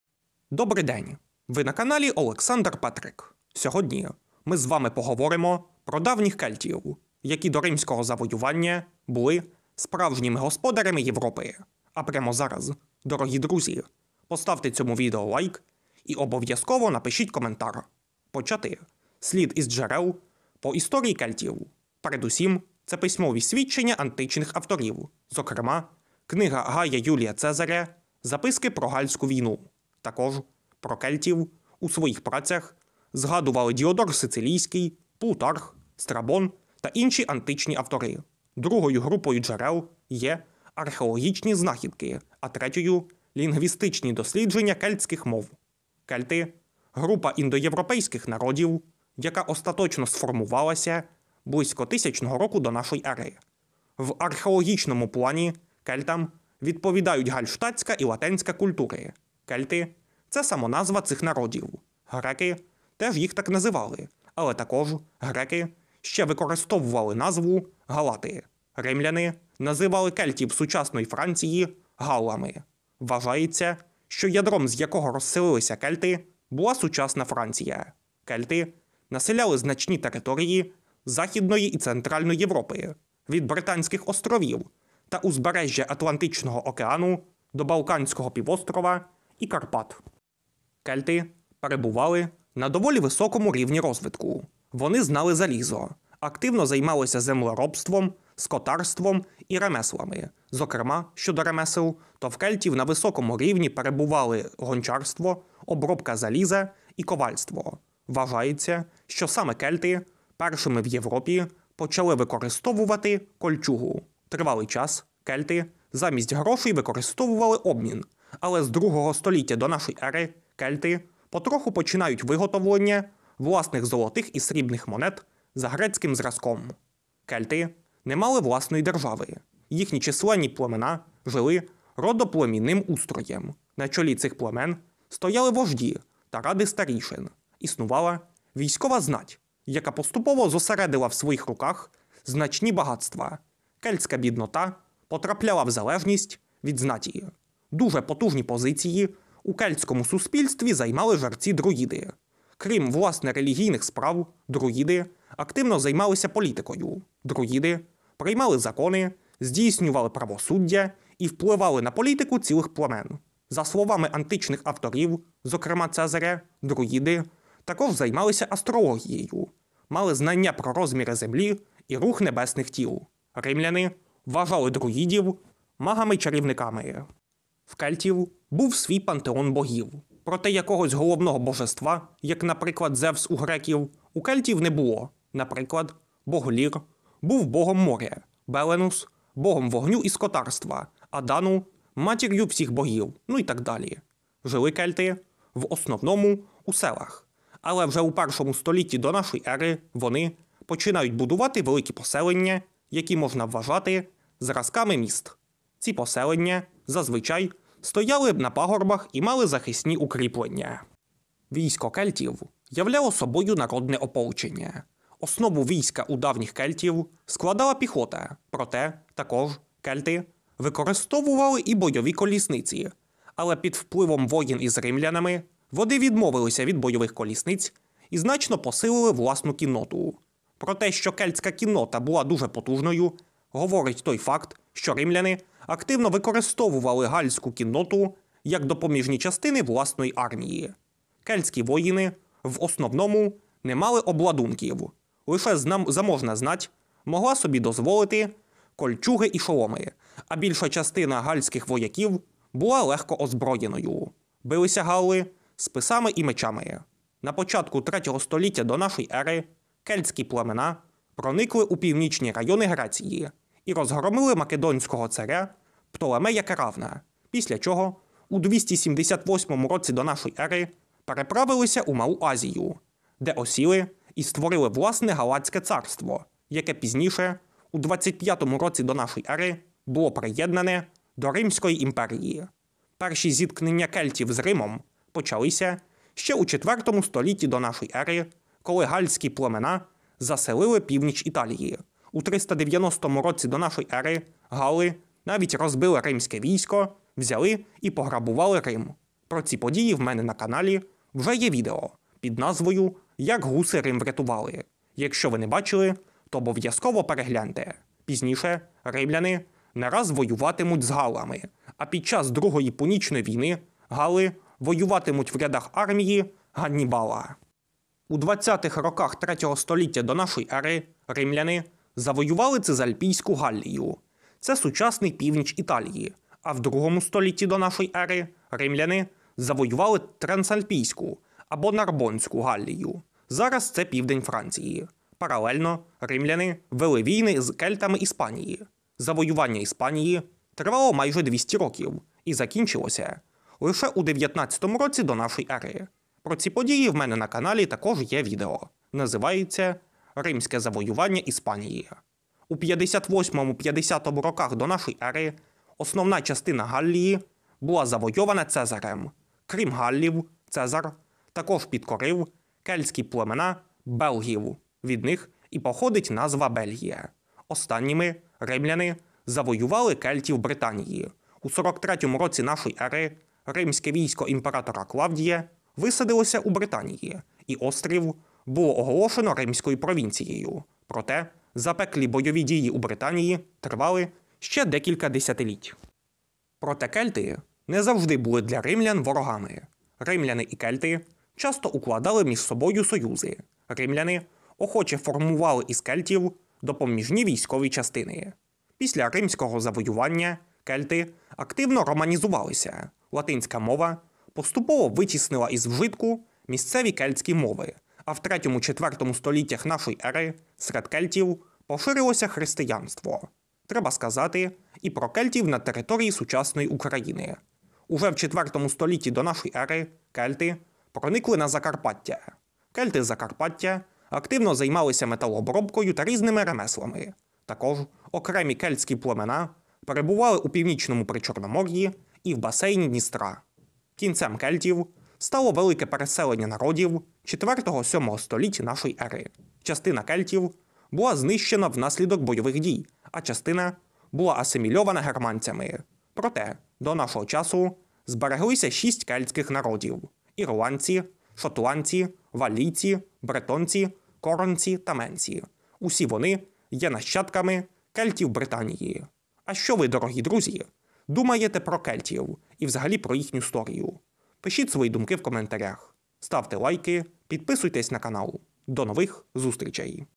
Кельти — давні господарі Європи
Кельти — група індоєвропейських народів, яка у I тисячолітті до нашої ери населяла значні території Західної та Центральної Європи.
00:00 Вступ
00:18 Джерела з історії кельтів
00:46 Хто такі кельти?
01:30 Господарство, економіка та політичний лад
02:28 Друїди та релігія кельтів
03:14 Поселення кельтів
03:31 Військо кельтів
04:17 Кельти на Балканах і в Малій Азії
04:42 Війни кельтів з римлянами (Галлія, Іспанія, Британія)
06:53 Кельти в Україні
07:14 Подальша доля кельтів